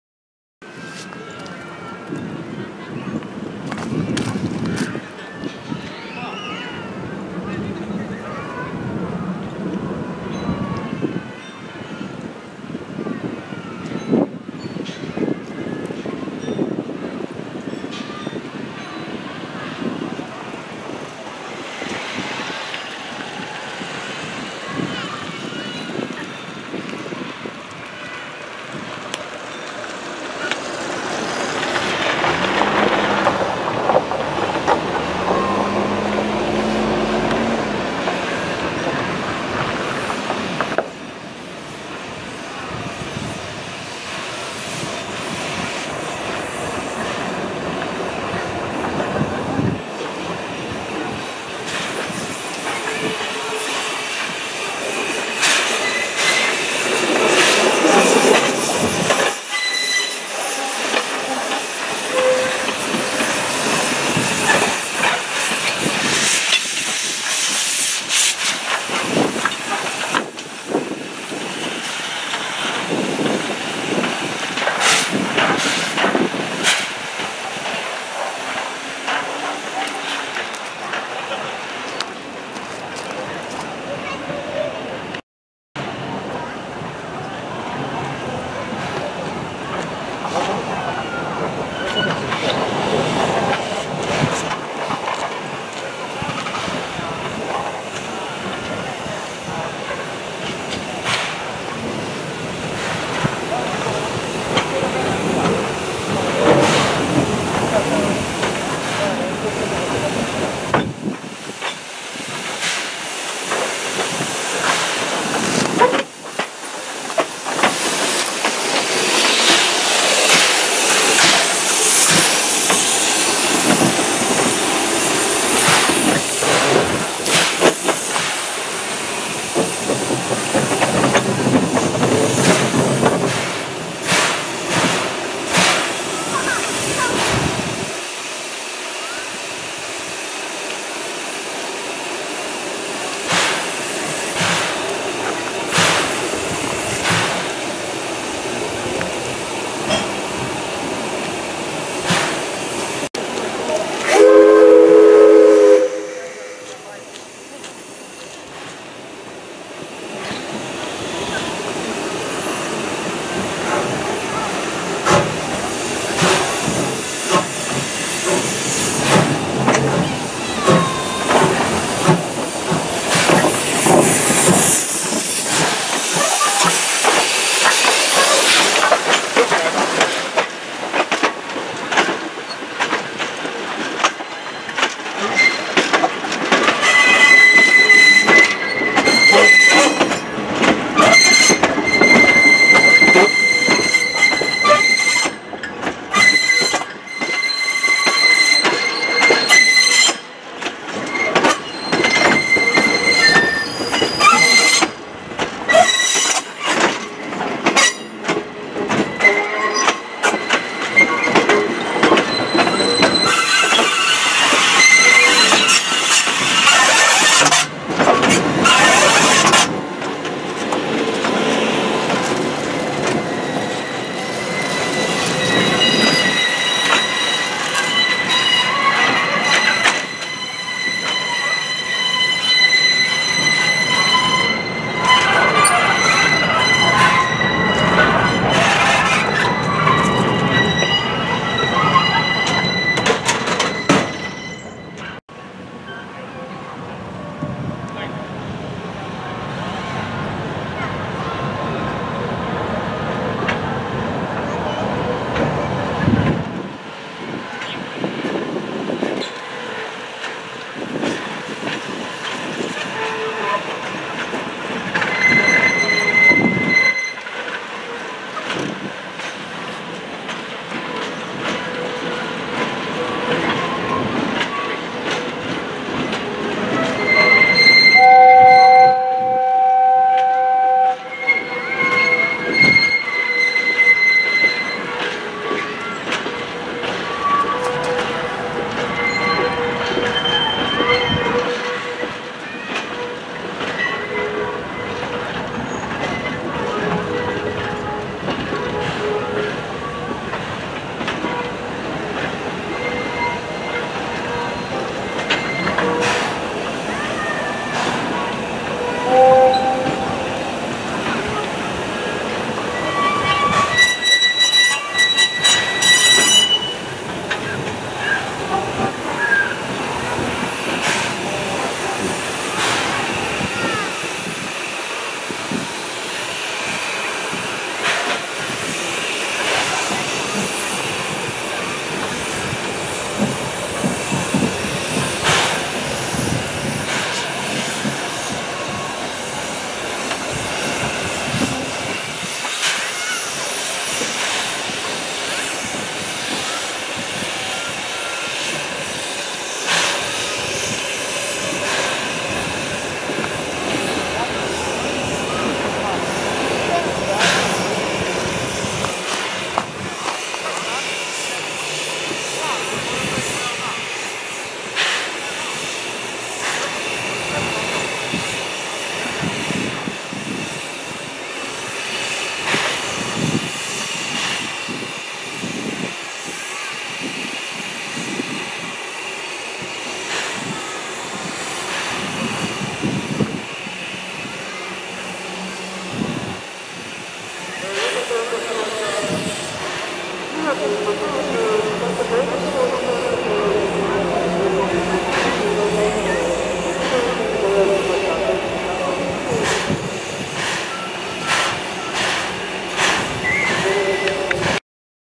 大井川鐡道トーマス千頭出発準備Feliz Navidad, Thomas Preparando por su Partida de Senzu, 19/Dec/2015
Season's Greetings from Thomas The Tank Engine. Thomas Festival 2015 at Senzu Station of Oigawa Railway, Shizuoka Prefecture, Japan. Thomas is preparing for departure from Senzu Station.
２０１５年１２月１９日トーマス号がクリスマス版で登場。川根本町の千頭駅にて。
Thomas, La Locomotora, versión Navideña en el año 2015. Solo esta semana trabaja Thomas entre la Estación Senzu y Shin-Kanaya de Ferrocarril de Oigawa en la Prefectura de Shizuoka, Japón. Thomas está preparando por su partida de la estación Senzu.
Kawane-Honcho, Shizuoka-Pref., Japan 19/December/2015